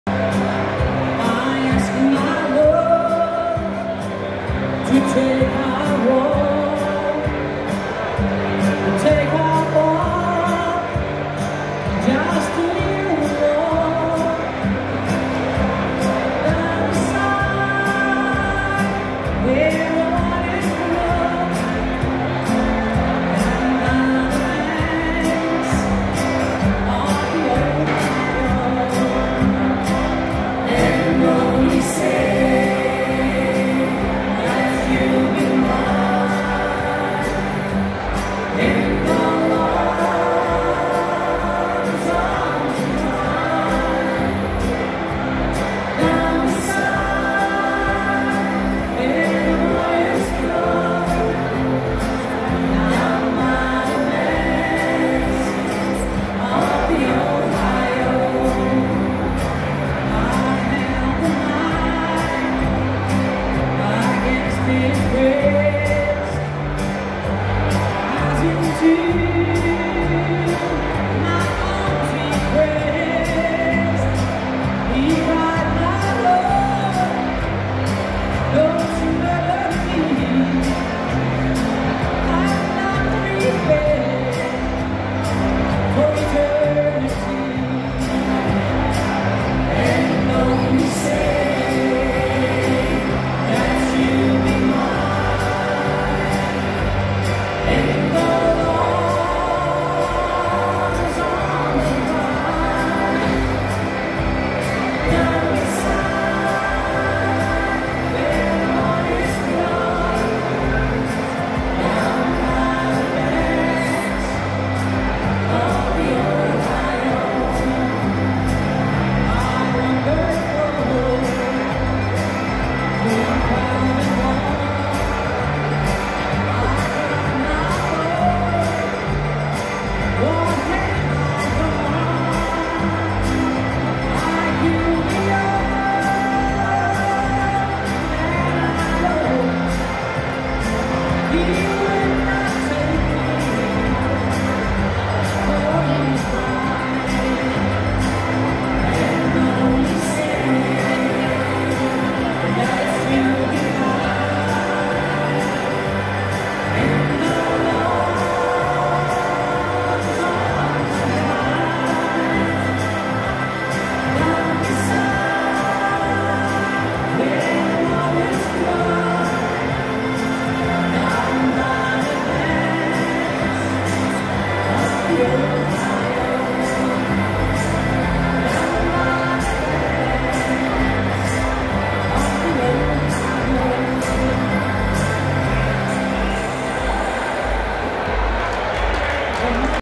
GRIET WIERSMA goes Country Flaeijelfeest 2023
in de grote tent voor de 20e keer zingt Griet op het Flaeijelfeest